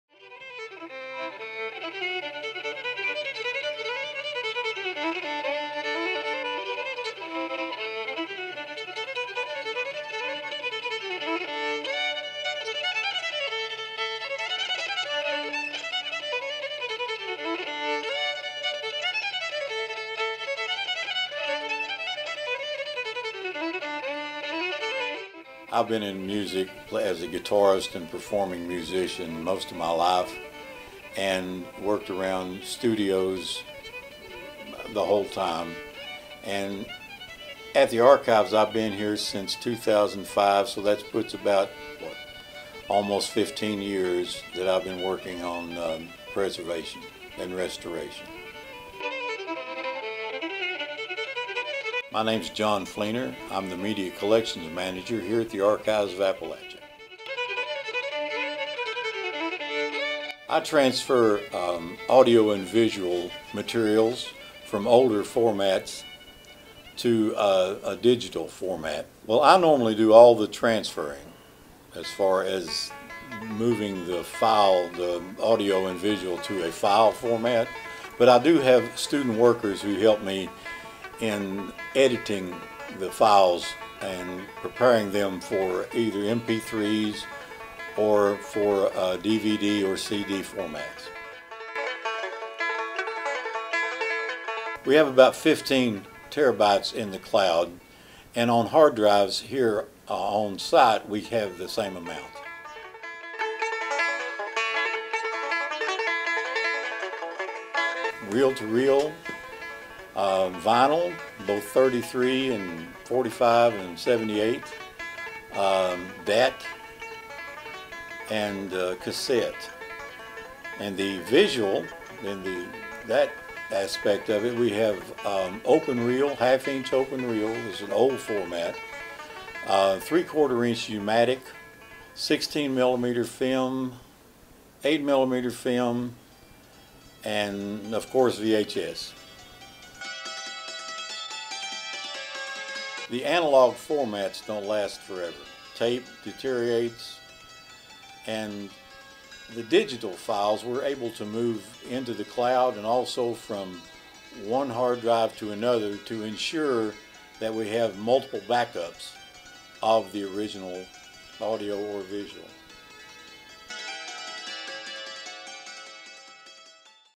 Media Preservation at the Archives of Appalachia
This video provides an overview of audio and moving image preservation efforts at the Archives of Appalachia at East Tennessee State University